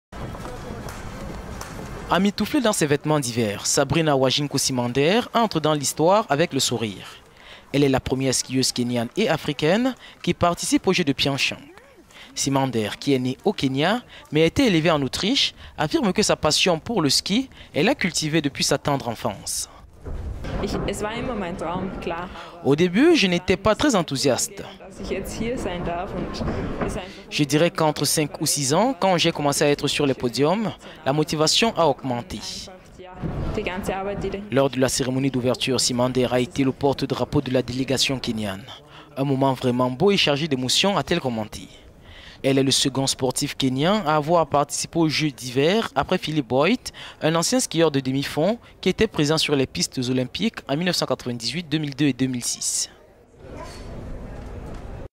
JO-2018 : la kényane Sabrina Wanjiku Simader vit son rêve
La Kényane Sabrina Wanjiku Simader, la première skieuse africaine à disputer les jeux Olympiques d'hiver 2018 de Pyeongchang dit vivre son rêve.  Elle s'apprête à se compétir sur les pistes de jeux de Pyeongchang.

Simader est née au Kenya mais a grandi en Autriche. Elle a déclaré que sa passion pour le ski augmentait à mesure qu'elle gagnait des courses lors de son jeune âge.

"Pour commencer, je n'étais pas si enthousiaste. Je dirais qu'à cinq ou six ans, quand j'ai commencé à être sur les po…